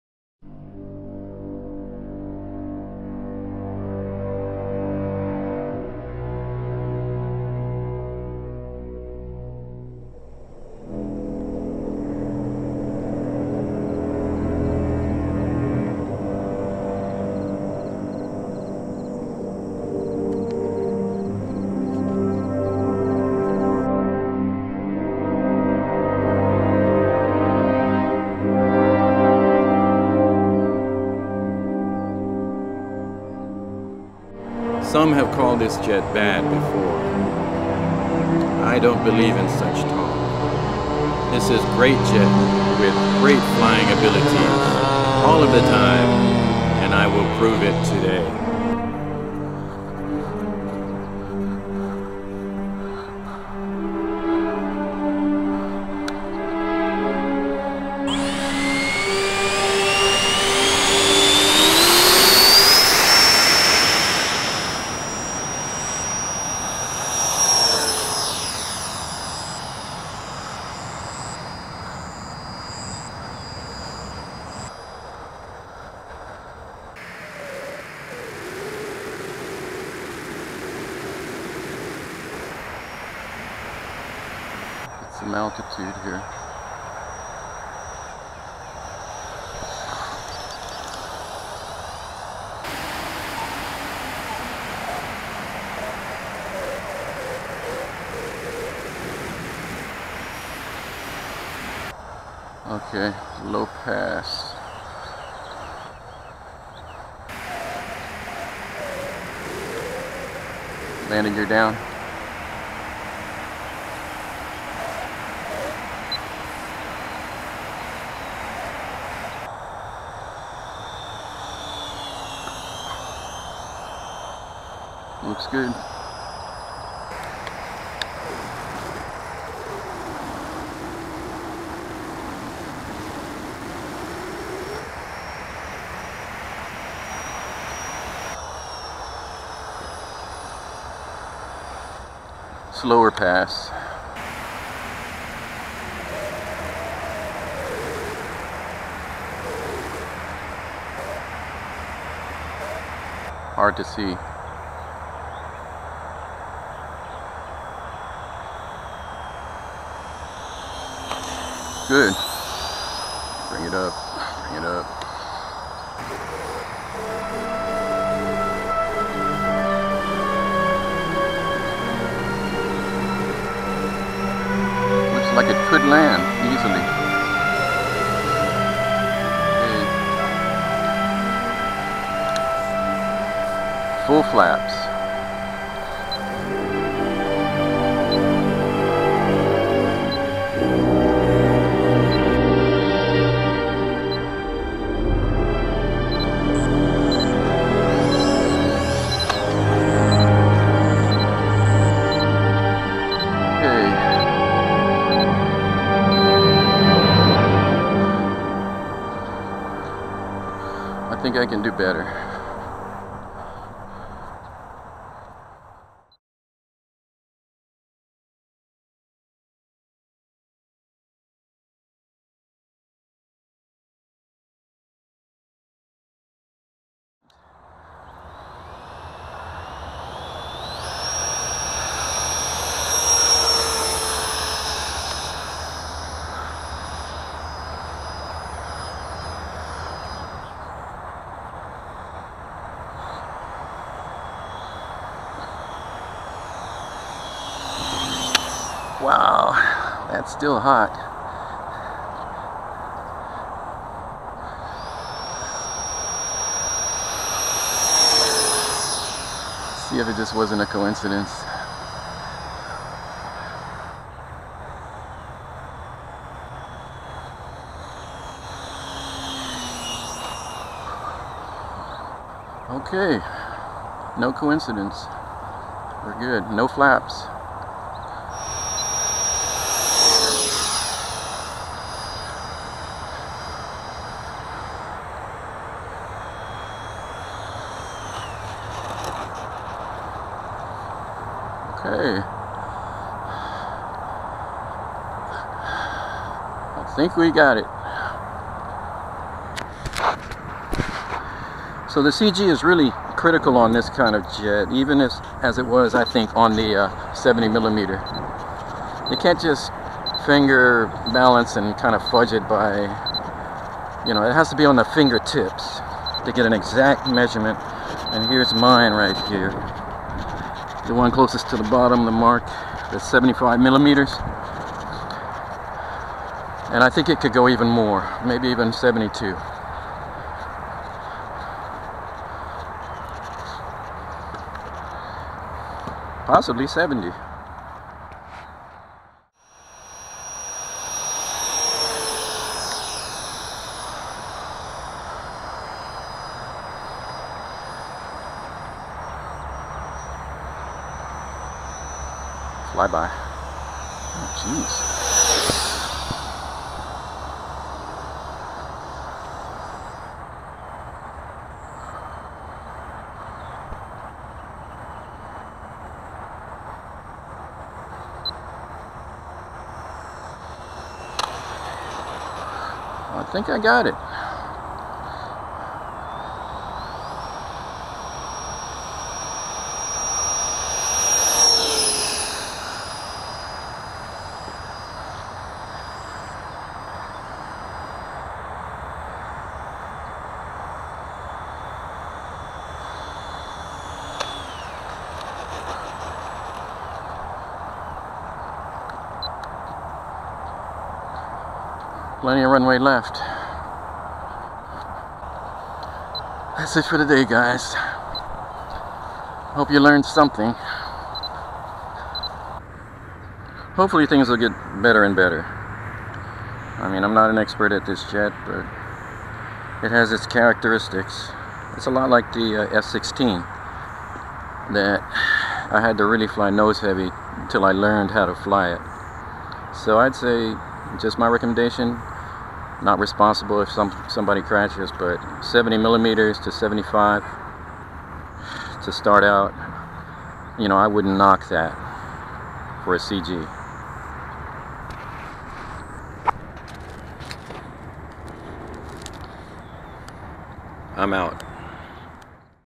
Freewing F-104 90mm Part 3
It was found that my last crash land into the water was due to an elevator disconnection in flight. The plastic part that connects directly to the elevator surface came apart, causing a loss of control. Because the jet pitched up when the elevator was disconnected lead me to discover that I had placed the CG too far aft causing the jet to be tail heavy. In this video I used a more nose heavy 75mm CG.